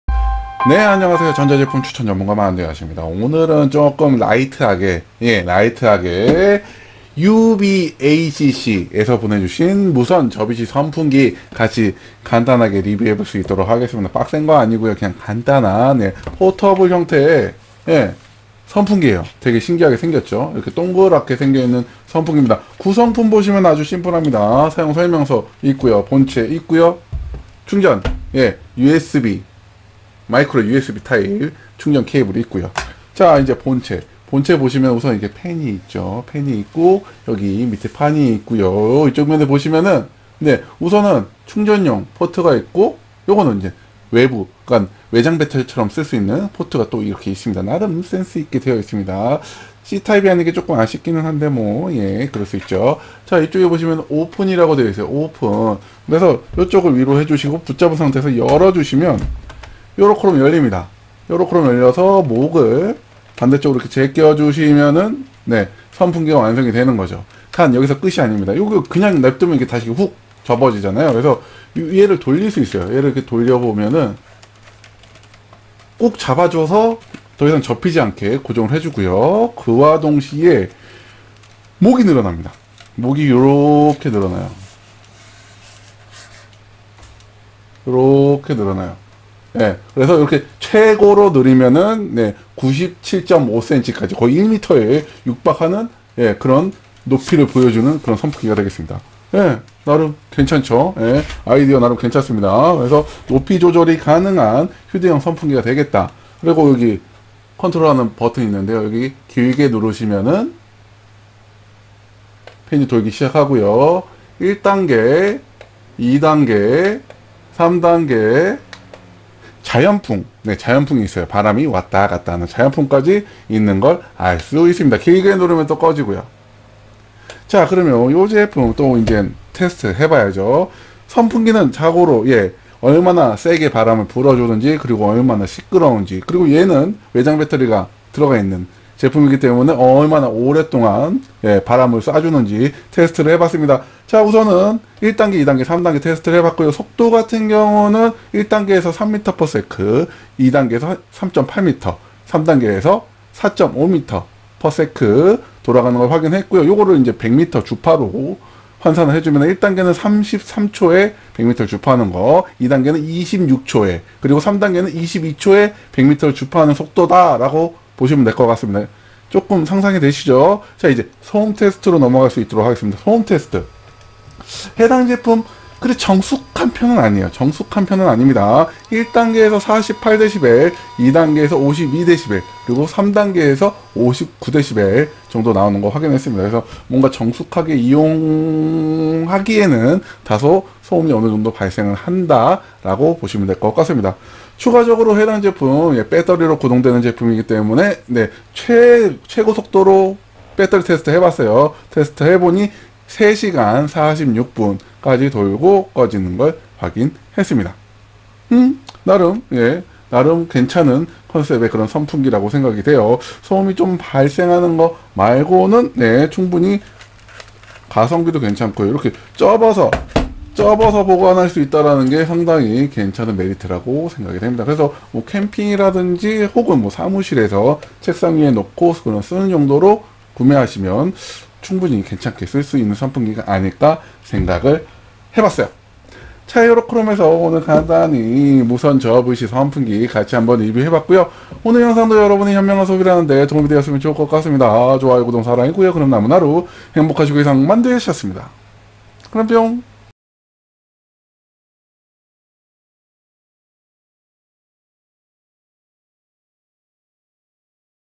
[리뷰] UBAcc 휴대용 무선 선풍기 - 키가 늘어나는 휴대용 선풍기
UBACC UBA-P7
상세 스펙
선풍기 / 탁상형 / 유무선 / 전원: USB /
[기능] DC모터 / 4단풍속 / 상하각도조절 / 무선기능 / 자연풍 / 캠핑겸용 / 그릴분리세척 / 접이식 /
[부가] 소비전력: 4.8W / 내장형충전지 / 배터리용량: 5200mA / 안전인증 / 소음: 40dB

이 포스팅은 제휴마케팅이 포함된 광고로 일정 커미션을 지급 받을 수 있습니다.
이 포스팅은 링크프라이스 이벤트 참여를 위해 작성되었습니다.